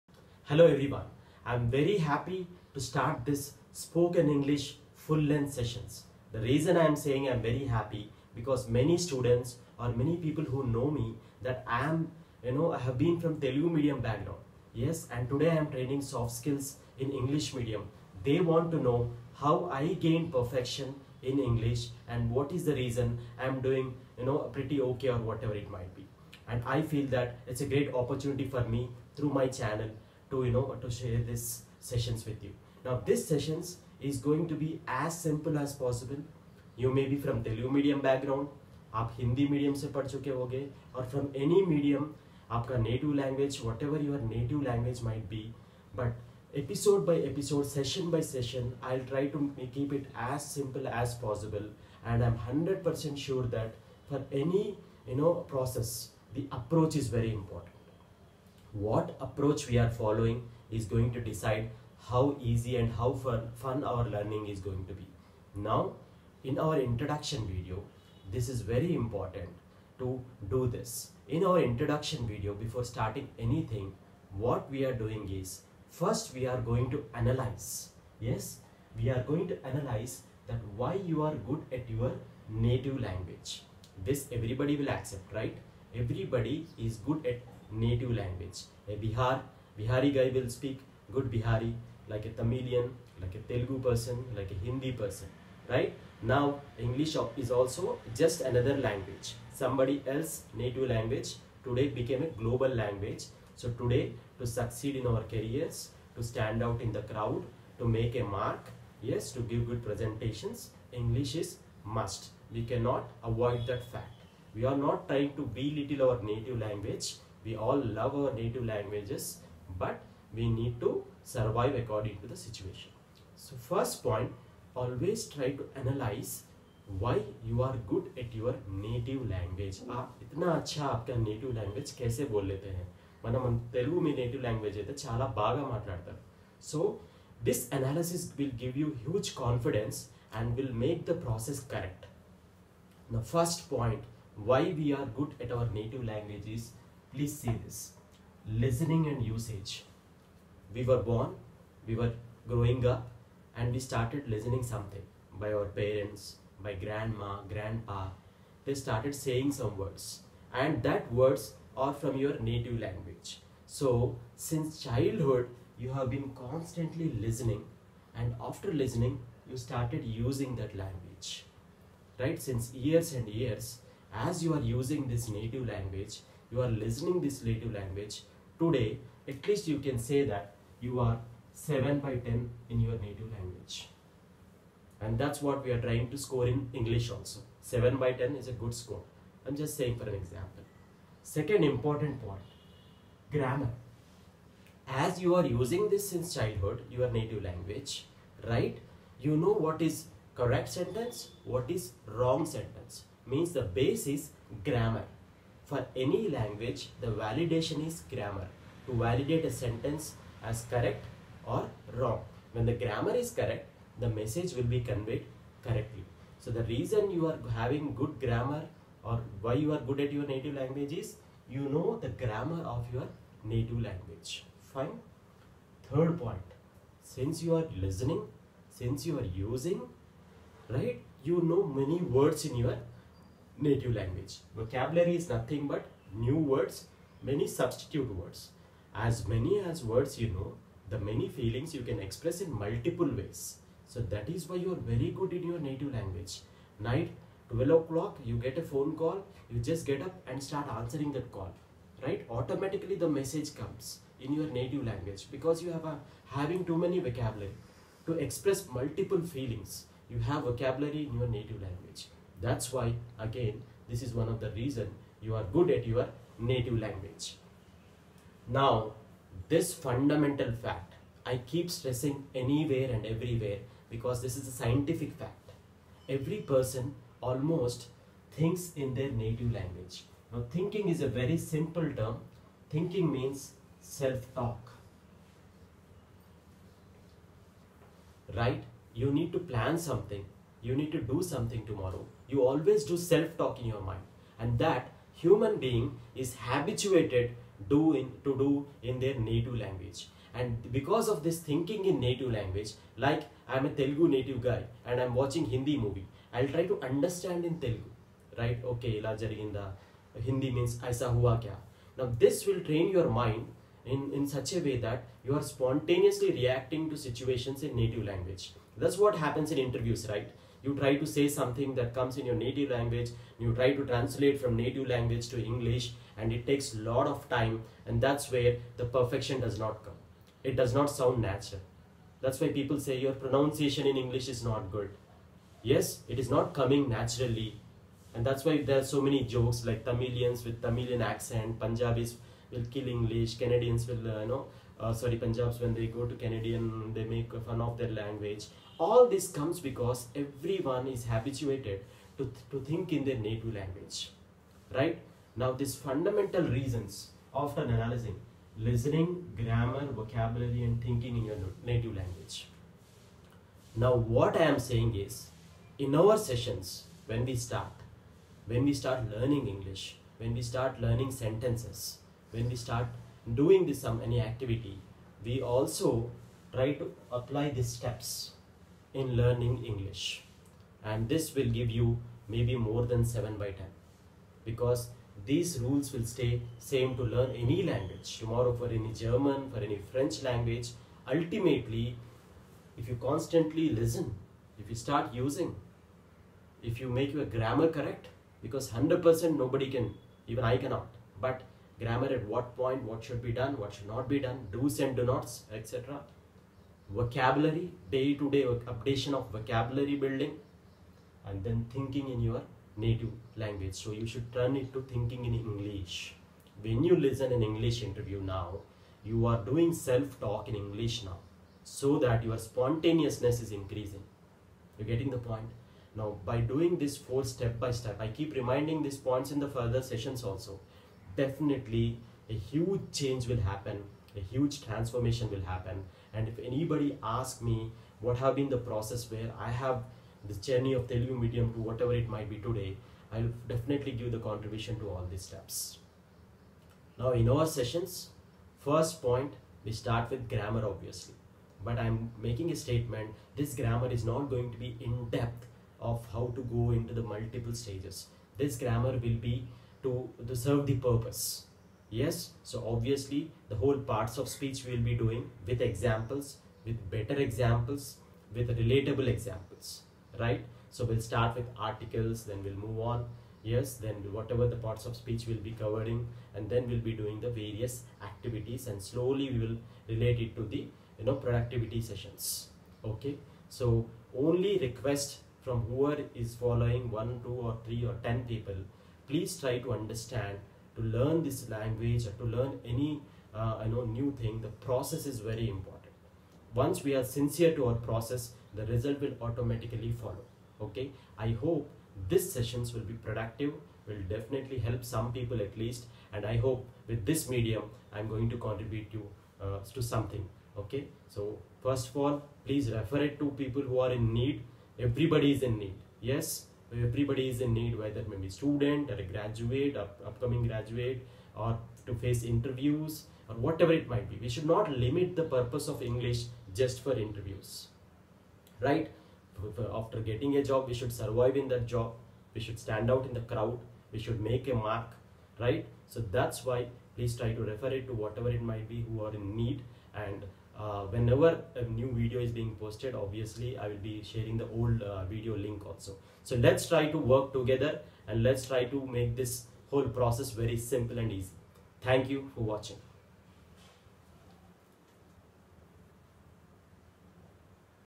Spoken English-Introduction
Learning a new language is an art! Discovering the fundamental reasons how one can gain a command on any new language like English